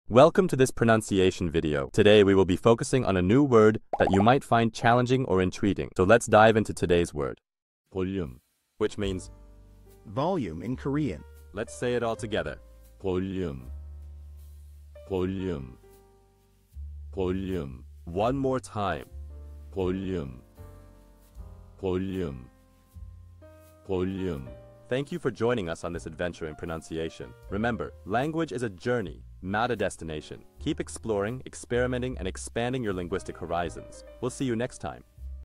How to pronounce bolryum | 볼륨 (volume in Korean)
Master the Pronunciation of 'bolryum | 볼륨' - which means : volume in Korean 📝 with @PronunciationKorean 🎓🗣️ - [Your Guide to Korean]

Learn to pronounce 'bolryum' perfectly in Korean with our easy and engaging tutorial by @PronunciationKorean! 📖📽️ In this comprehensive video, we provide you with native speaker examples 🗣️👂 and phonetic breakdowns 🎵 to ensure you get the pronunciation just right.